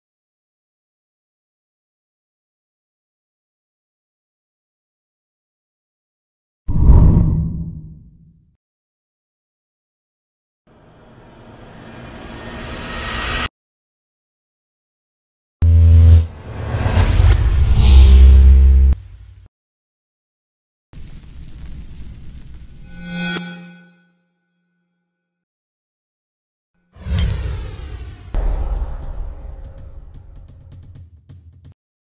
Logo Intro ( After Effects Template Intro )
Easy to use. Just drag and drop your logo

4k and Full HD Resolution

Color controller for shapes

Video Tutorial

5 Logo Intro animation

Sound Effect included

Total File size 249 Including Video tutorial (After Effect Project 23.5 Mb)
All Playlists Videhive After Effects Template:
Other categories of products:
Audiojungle - Stock Music and Audio: